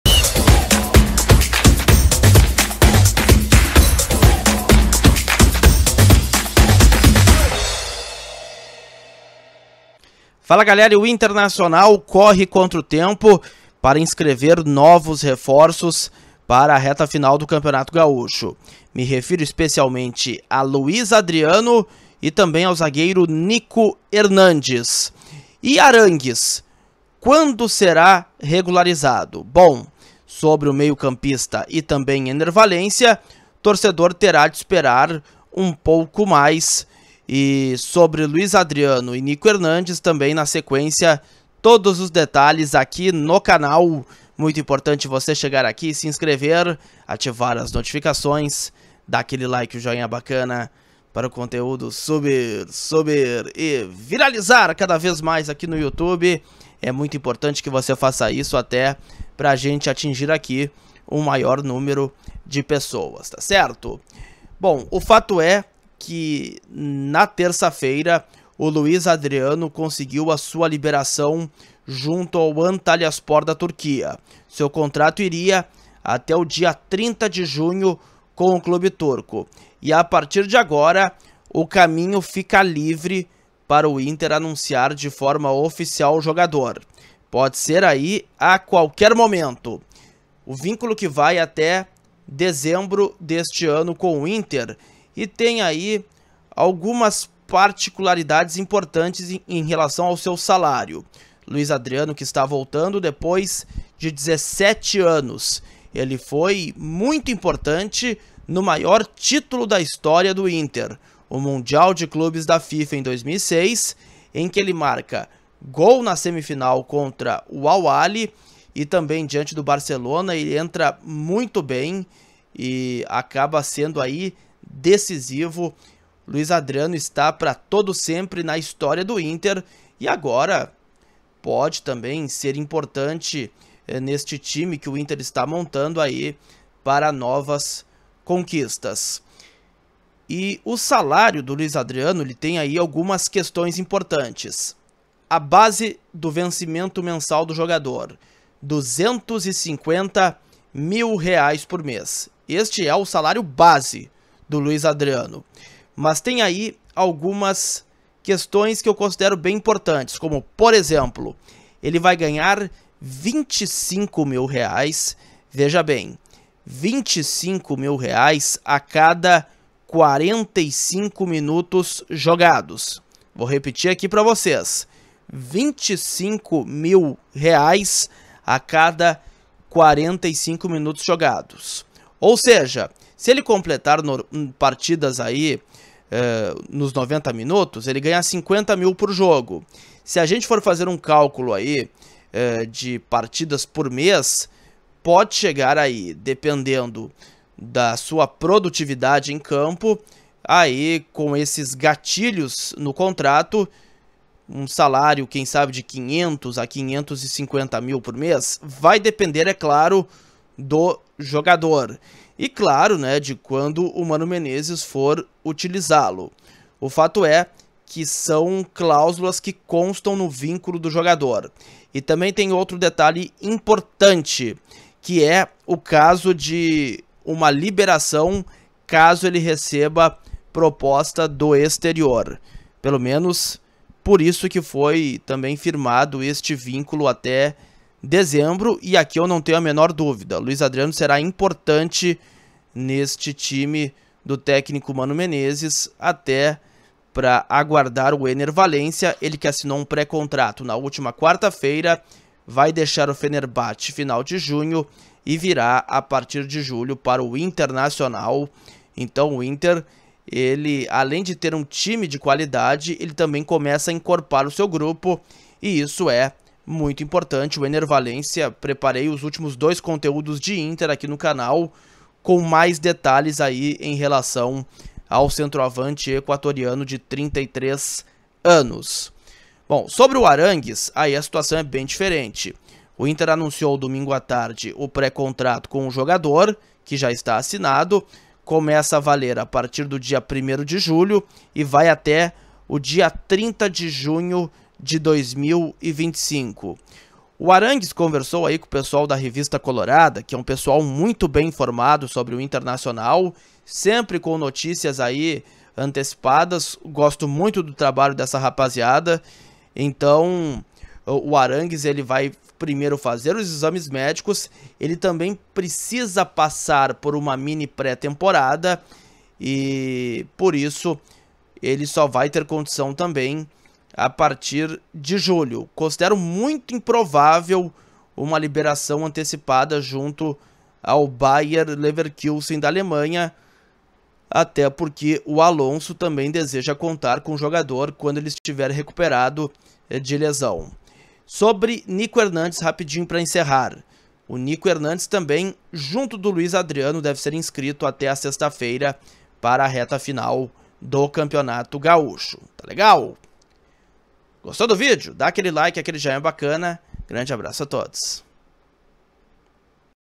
INTER: PRAZO PARA INSCREVER LUIZ ADRIANO NO GAUCHÃO | ANÚNCIO IMINENTE | E ARÁNGUIZ?
Terça-feira muito movimentada no Inter. Te explico em detalhes no vídeo.